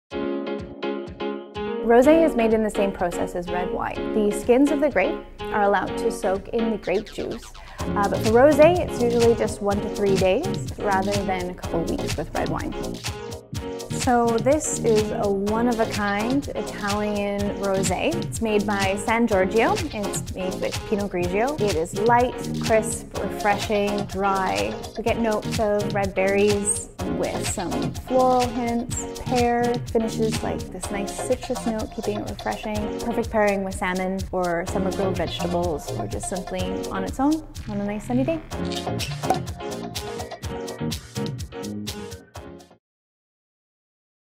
An Intro to Rosé with Kady Smith
Ever wondered how rosé was made? Ever wondered the best meals to pair with this summertime favourite? Dive into the world of rosé with Kady Smith - a WSET trained staff member at Everything Wine!